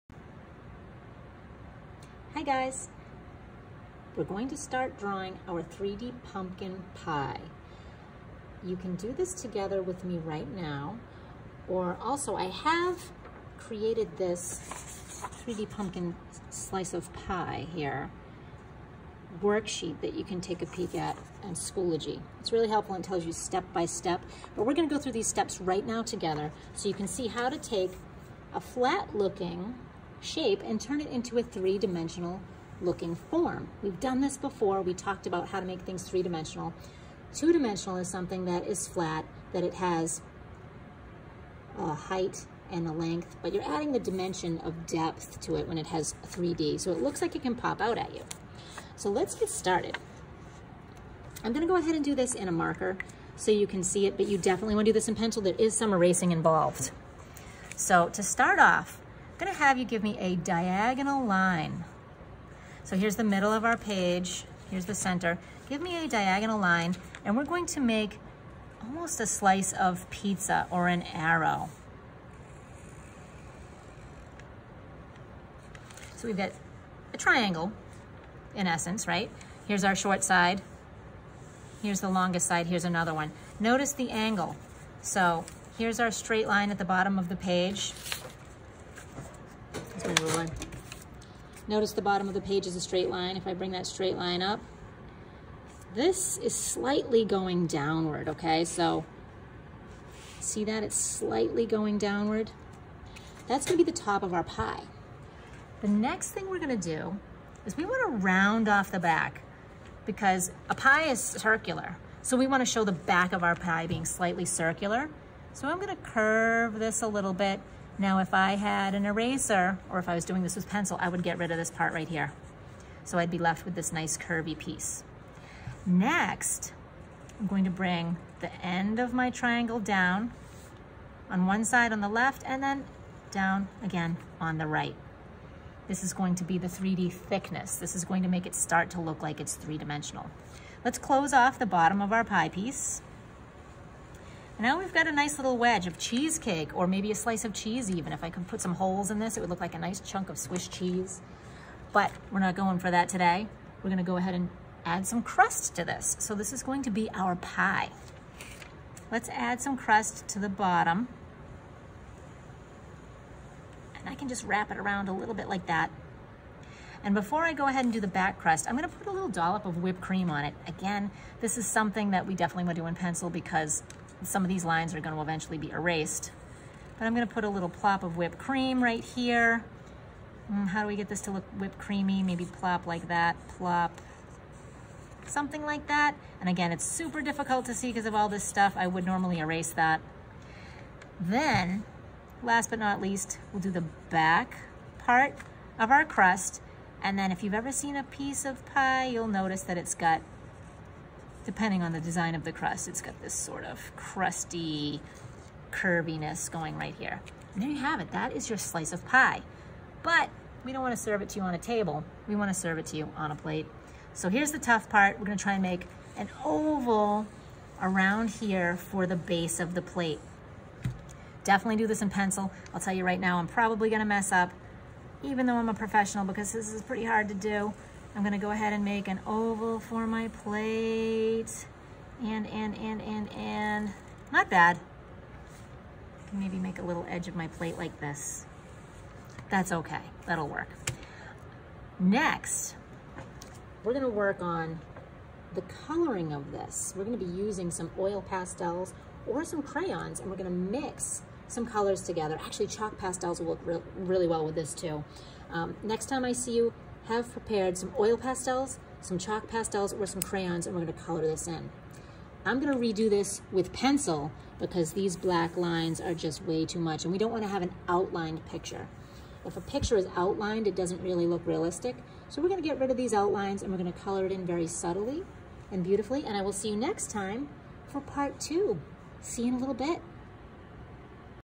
3D Pumpkin Pie Part 1
In this lesson students will learn how to draw a three dimensional slice of pie.